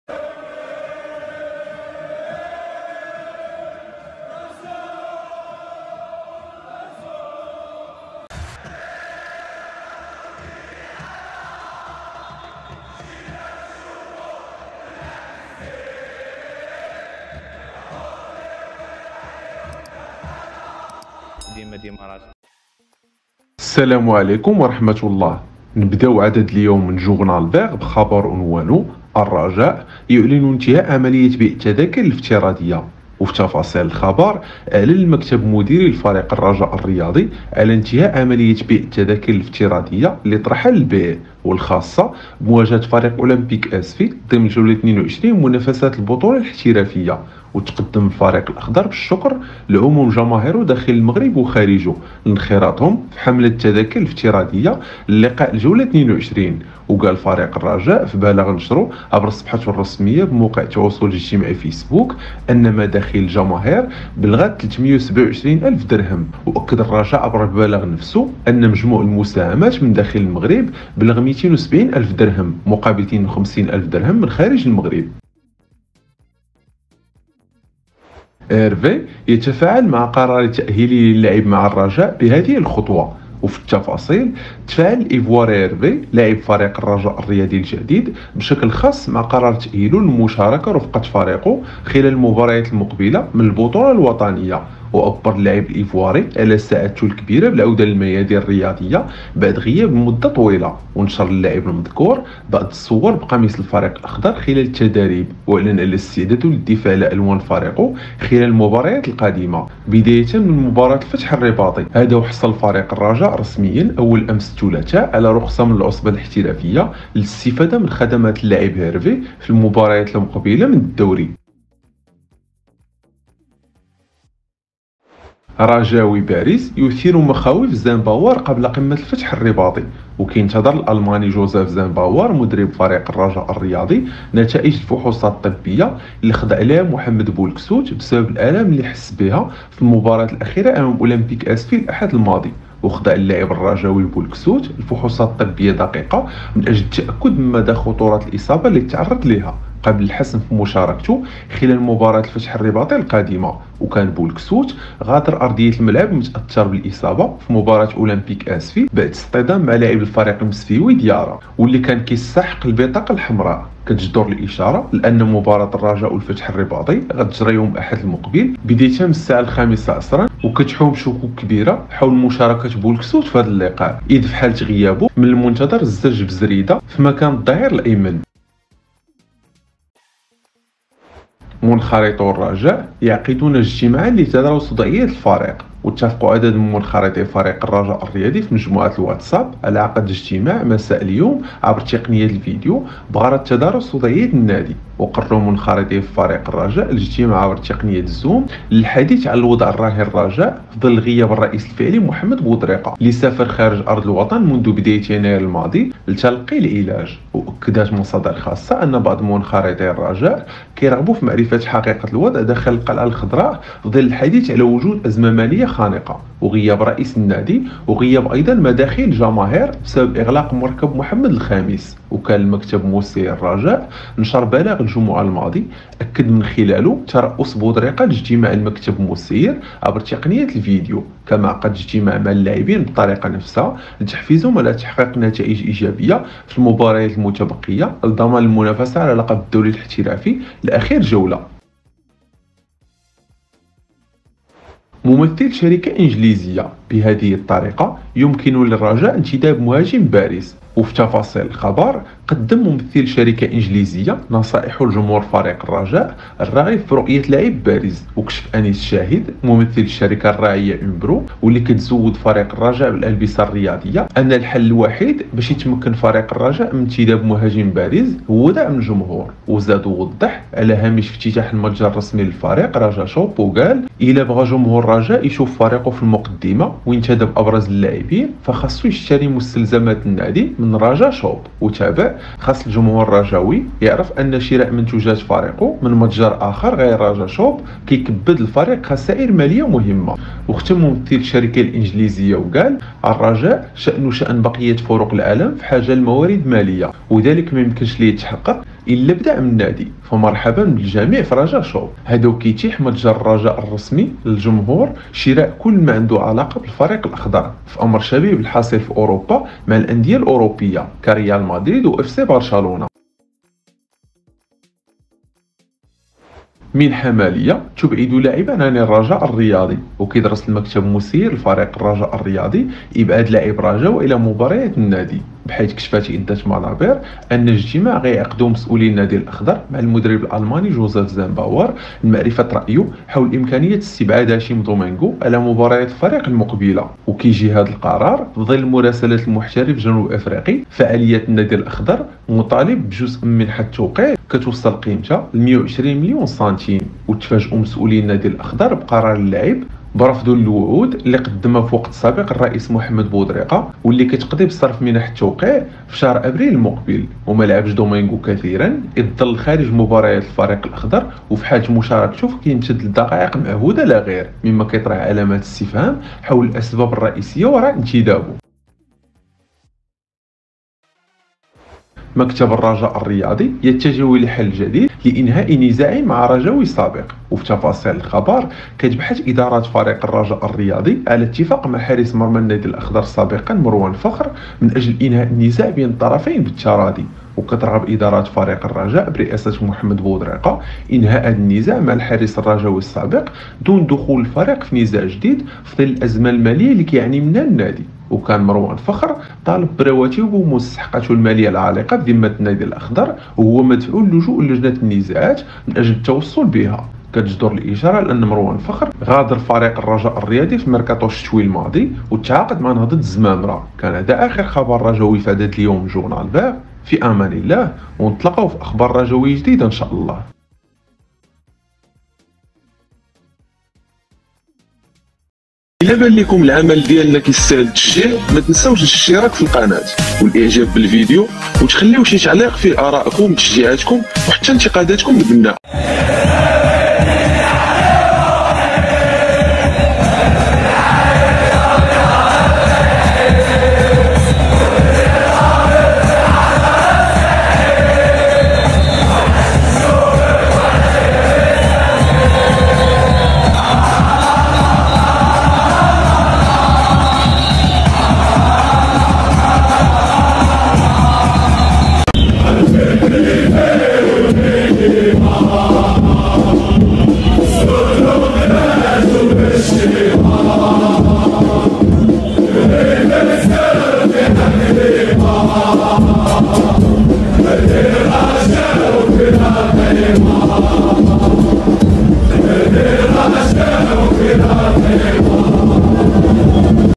انتداب مهاجم بارز للرجاء | رد فعل هيرفي بعد التأهيل | زينباور ينتظر خبرا مهما | قرار جديد في حق هاشم
جوغنالڤيغ  لهذا اليوم يتضمن آخر الأخبار الرجاوية من إعداد طاقم القناة و التي جاءت في الفيديو كاتالي :

ـ الرجاء يعلن انتهاء عملية بيع التذاكر الافتراضية ـ
ـ هيرفي يتفاعل مع قرار تأهيله للعب مع الرجاء بخطوة مميزة ـ
ـ رجاوي بارز يثير مخاوف زينباور قبل قمة الفتح الرباطي ـ
ـ منخرطو الرجاء يعقدون اجتماعا لتدارس وضعية الفريق ـ
ـ ممثل شركة إنجليزية: بهذه الطريقة يمكن للرجاء انتداب مهاجم بارز ـ
ـ منحة مالية تبعد لاعبا عن الرجاء الرياضي ـ
ـ الرجاء يتجه لحل جديد لإنهاء نزاع مع رجاوي سابق ـ

لا تنسوا الإشتراك فالقناة و تفعيل الجرس و ترك تعليقاتكم على الفيديو لكي يصلكم الجديد أولا بأول.

فرجة ممتعة.